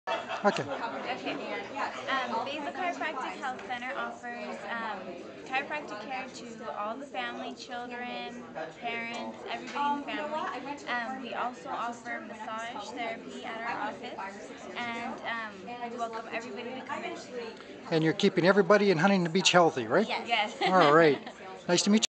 The Basil Chiropractic Girls give a plug
The girls from Basil Chiropractic give Dr Basil a plug at the Winners Circle Breakfast